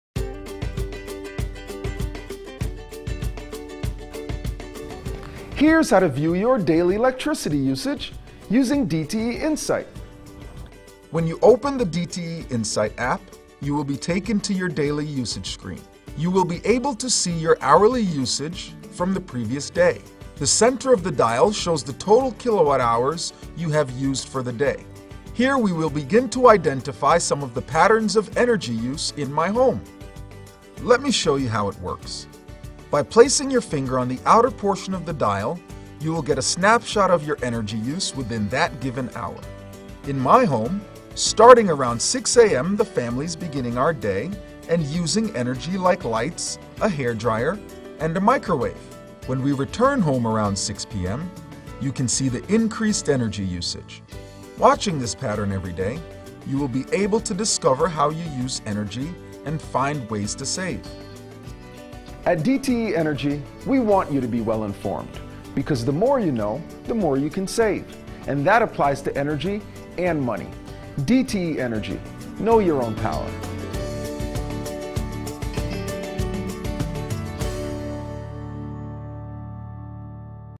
How to view your daily electricity usage without an energy bridge
DTE Energy Insight APP video on how to view your daily electricity usage without an energy bridge.